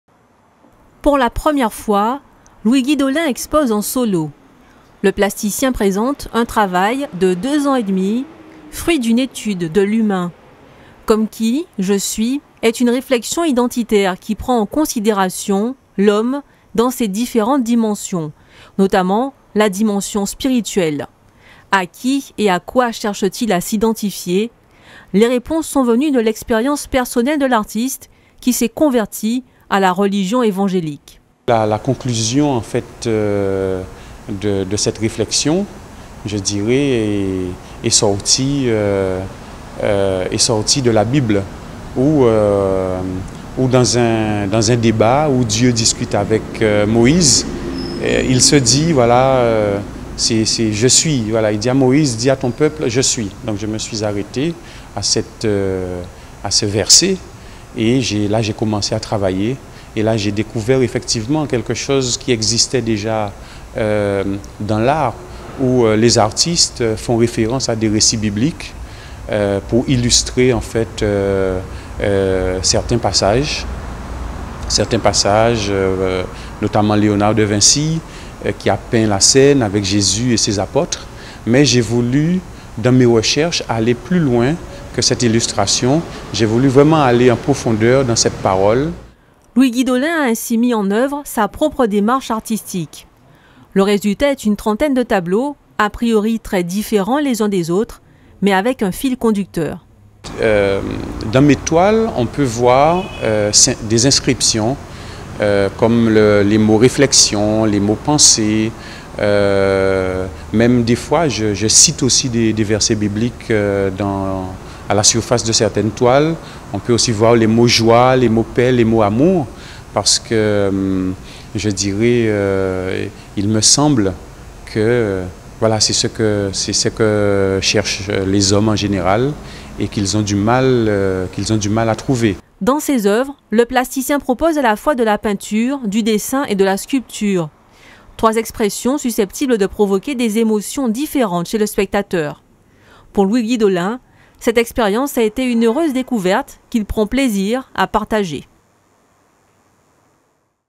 Exposition de Louis-Guy Dollin
Jusqu’au 31 décembre, Louis-Guy Dollin présente une série d’une trentaine de tableaux au centre culturel Rémy Nainsouta à Pointe-à-Pitre. A travers sa première exposition personnelle baptisée « Comme qui, je suis », l’artiste et professeur d’arts plastiques propose sa vision du monde.